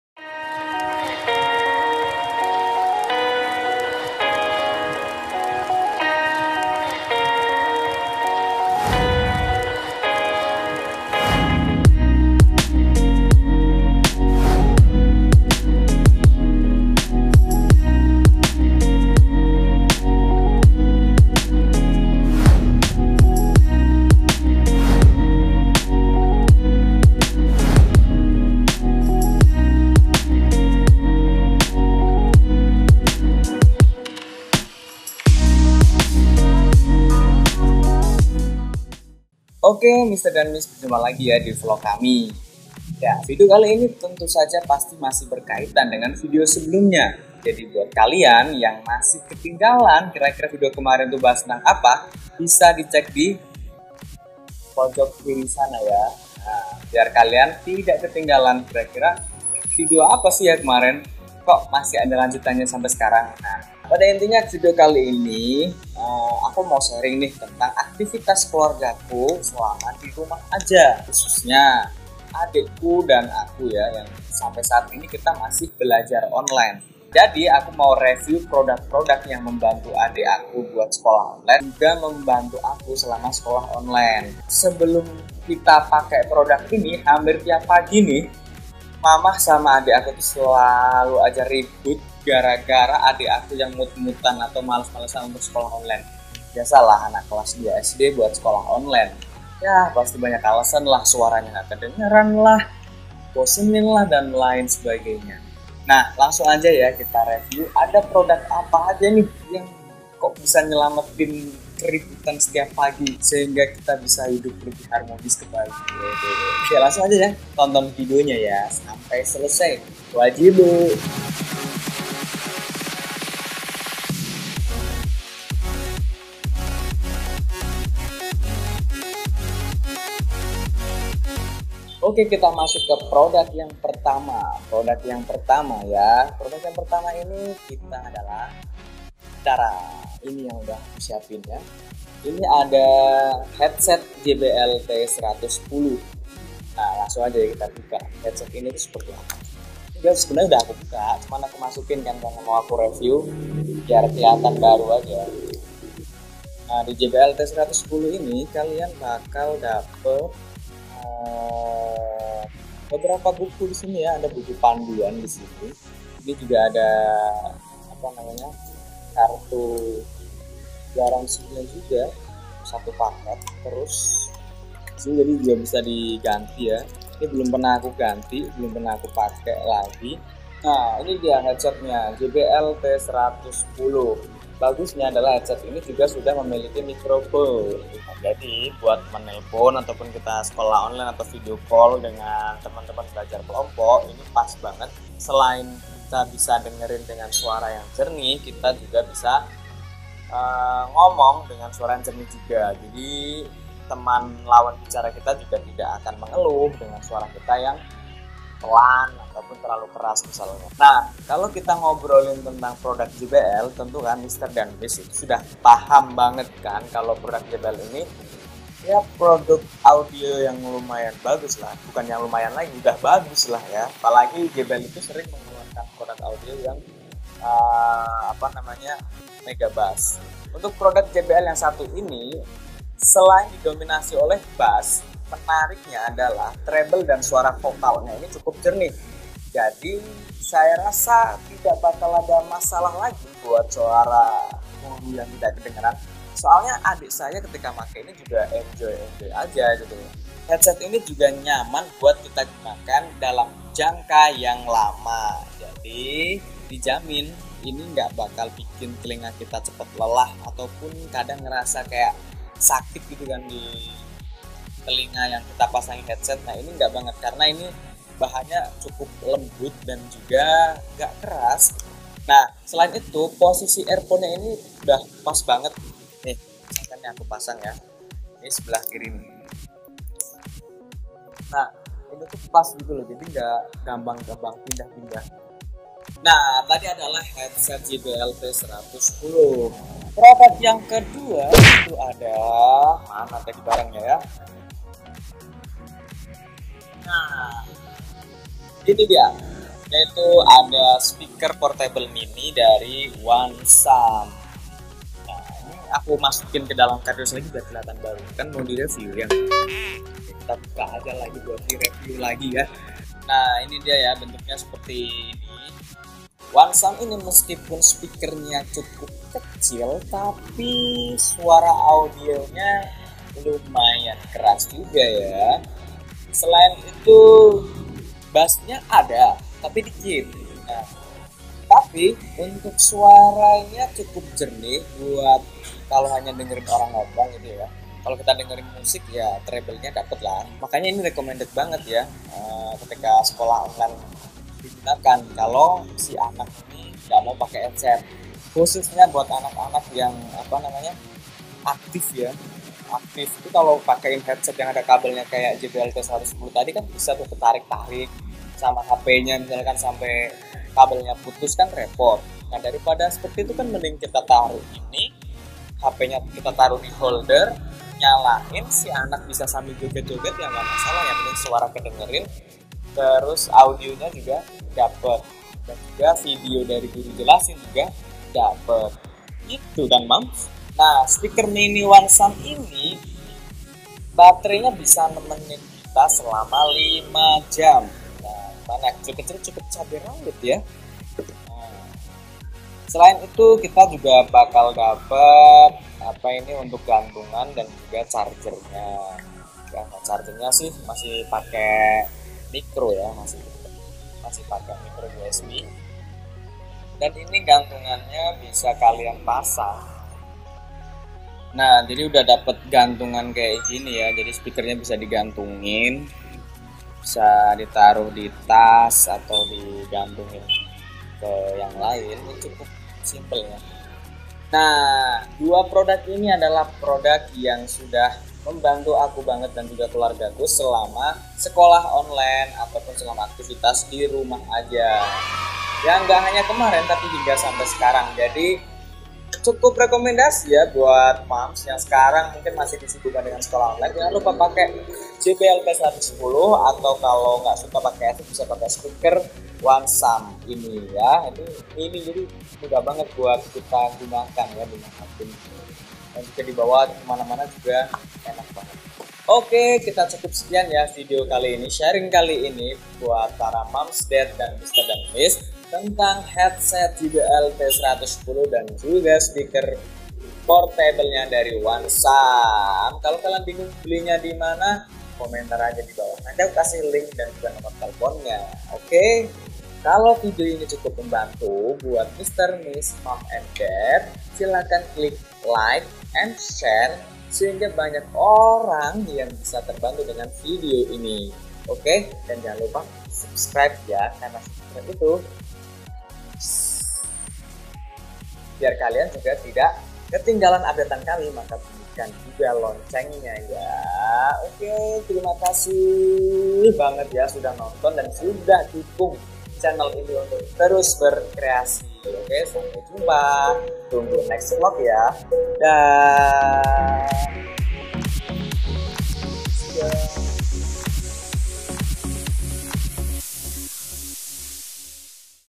Vlog #9 Headset dan Speaker TerRECOMENDED!!
Vlog kali ini sharing tentang headset dan speaker terRECOMENDED pokoknya buat kita yang bergelut dengan sekolah online. Ini produk gak sembarangan loh aktivitasnya, jadi gak bakalan kecewa dah.

biar tahu produknya apa dan belinya dimana, tonton aja videonya langsung yah. kalo ada yang ingin di tanyain atau kritik dan saran tulis di komentar yah biar kami bisa terus berbenah dan memberi yang terbaik buat mr dan misss.

Yang mau cek aksesoris gadget langsug aja cek dan follow Instagram kami disini :

Yang mau update promo langsug cek dan follow Instagram official kami disini